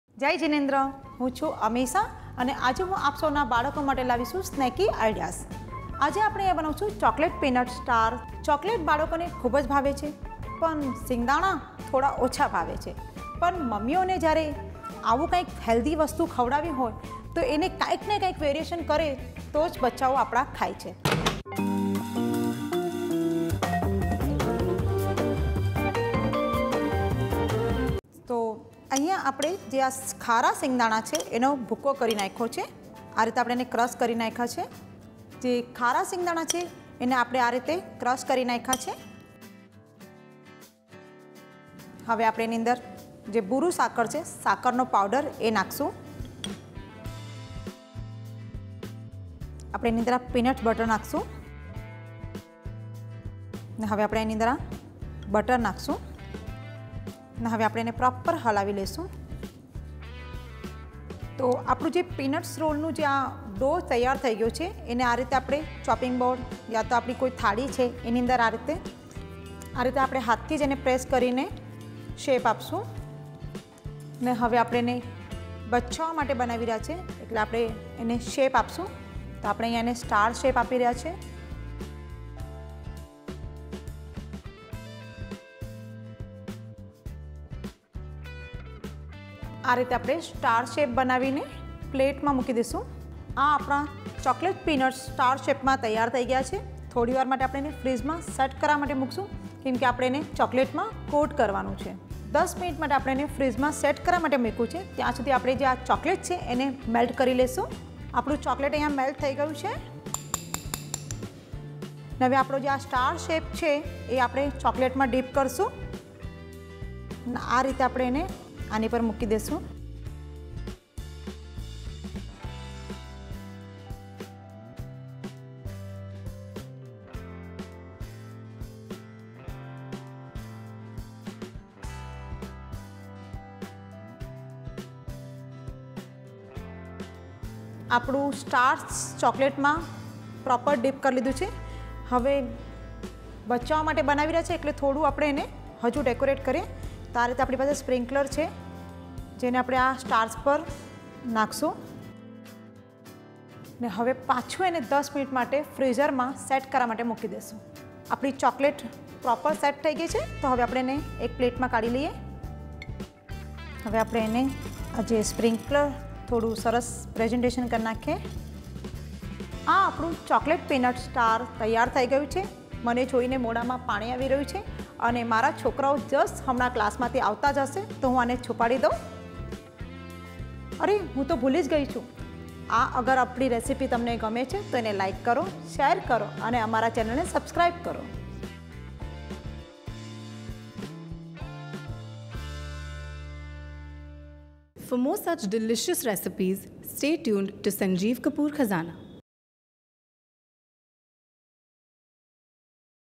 Chocolate Peanut Stars In Gujarati | Snacky Ideas by Amisha Doshi | Sanjeev Kapoor Khazana
Crushed peanuts mixed with sugar, butter, shaped into stars and dipped into chocolate. Watch this video to find out how to make this recipe.

CHOCOLATE PEANUT STARS

Ingredients

1 cup chopped dark chocolate
¾ cup salted crushed peanuts
2 tablespoons powdered sugar
1 tablespoon butter + for greasing
1 tablespoon peanut butter
Edible colourful sprinklers for sprinkling

Method

1. Take crushed peanuts in a bowl. Add powdered sugar, 1 tablespoon butter and peanut butter and mix till well combined.
2. Flatten the peanut mixture on the worktop to make a thick sheet and cut into stars using a small star cookie cutter. Transfer on a plate and refrigerate for 10 minutes.
3. Take dark chocolate in a microwave proof bowl and melt in microwave for 1 minute.
4. Coat the peanut stars with melted chocolate, place on a rack and sprinkle some colourful sprinklers on top. Refrigerate for 10 minutes or till chocolate sets.
5. Serve.